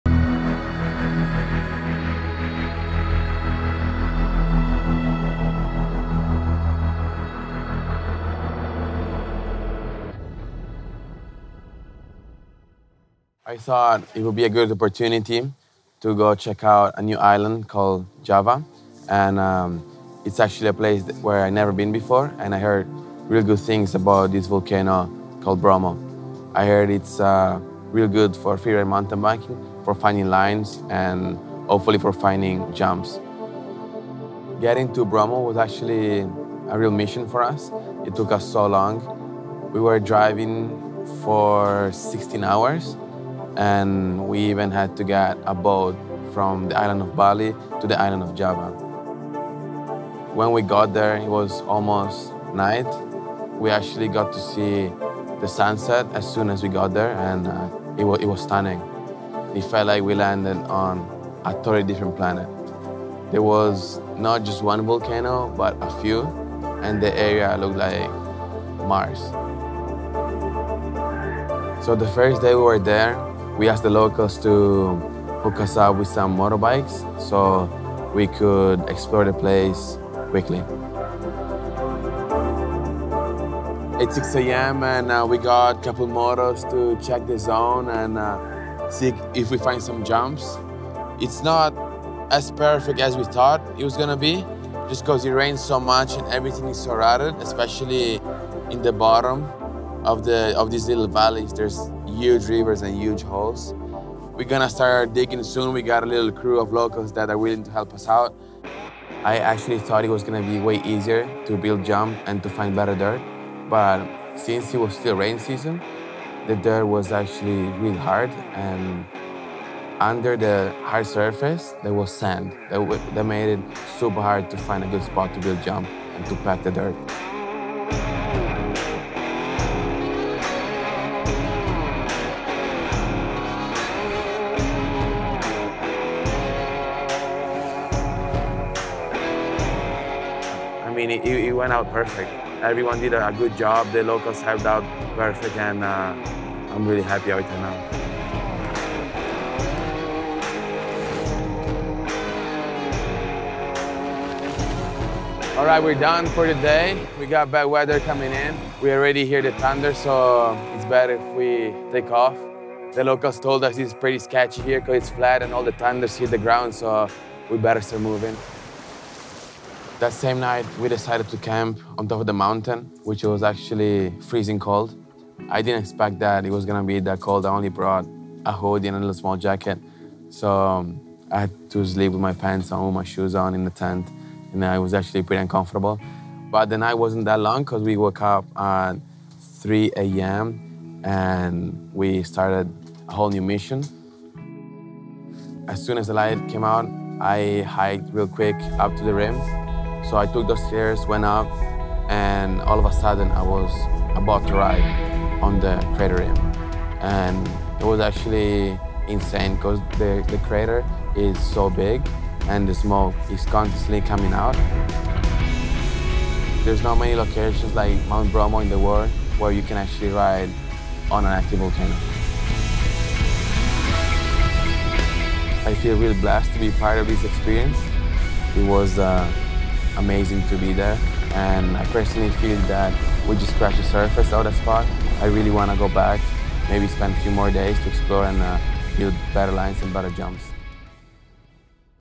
Riding Down An Active Volcano On A MTB
Italian freeride mountain bike rider @NickPescetto on an epic quest to ride an active volcano in Java 🌋